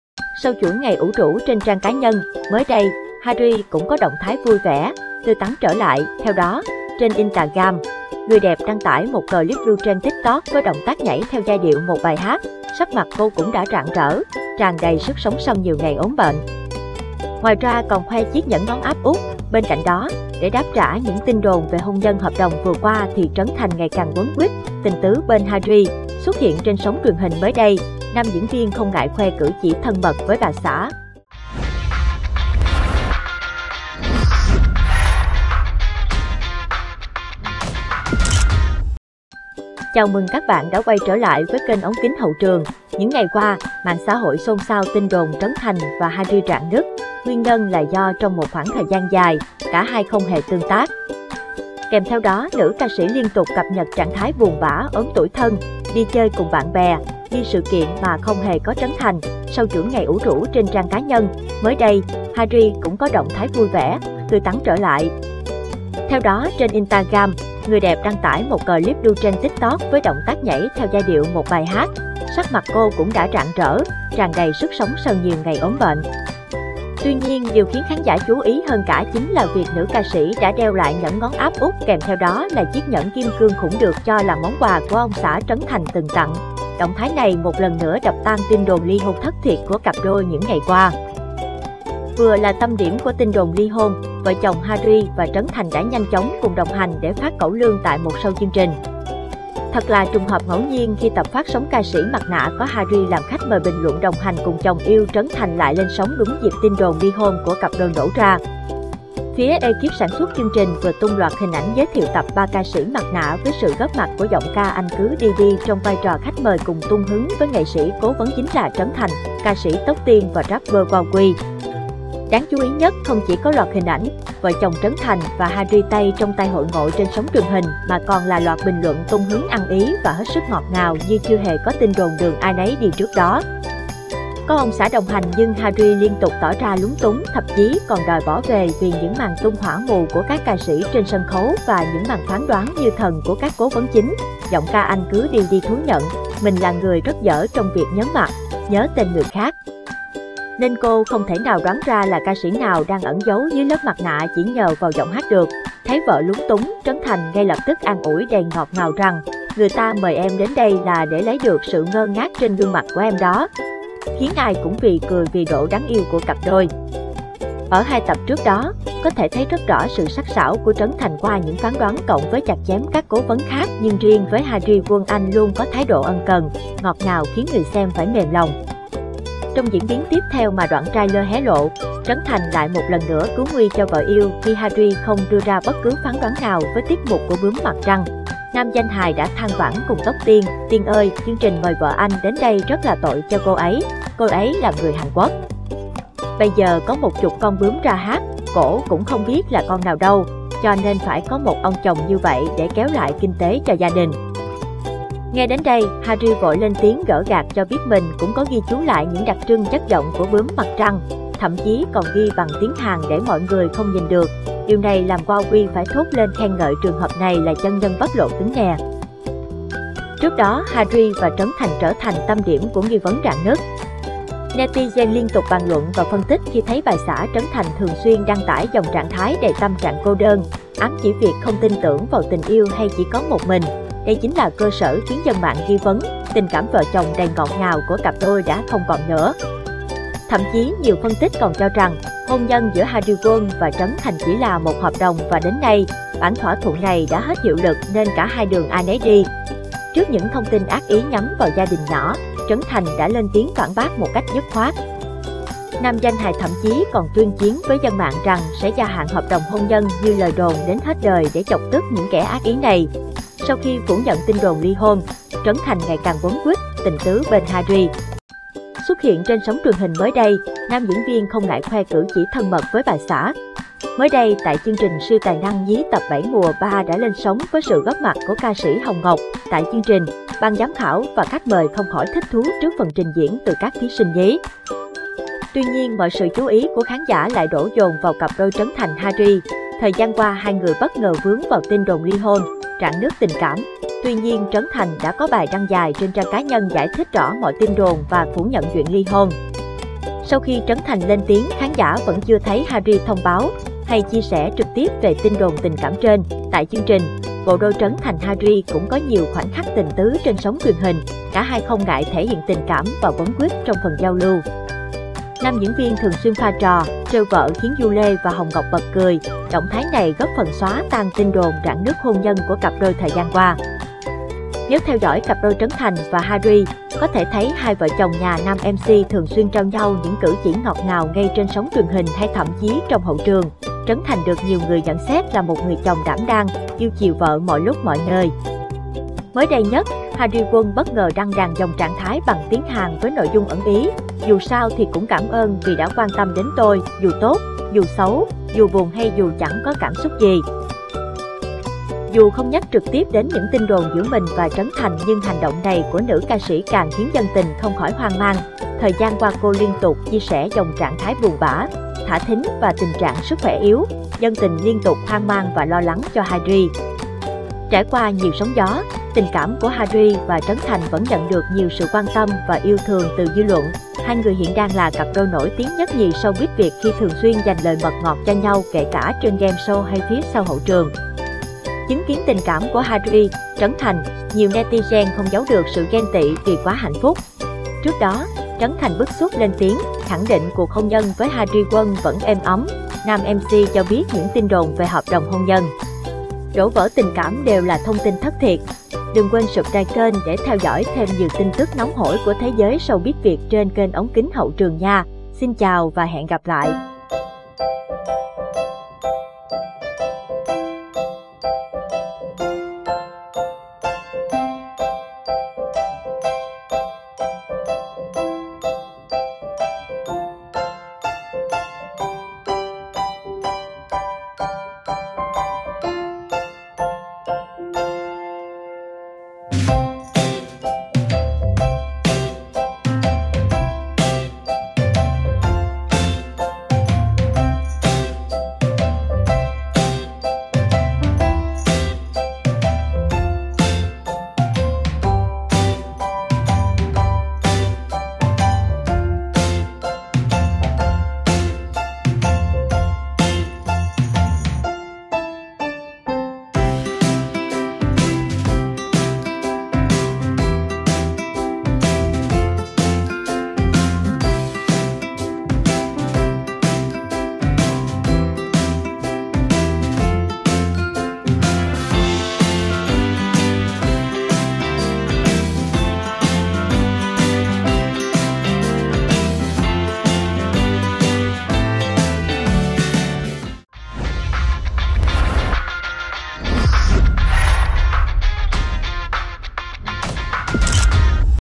Vợ Trấn Thành khoe NHẪN KIM CƯƠNG BẠC TỶ, ÔM ẤP THÂN MẬT ĐÁP TRẢ cực gắt tin đồn hợp đồng hôn nhân
Sau chuỗi ngày ủ rũ trên trang cá nhân, mới đây, Hari Won cũng có động thái vui vẻ, tươi tắn trở lại.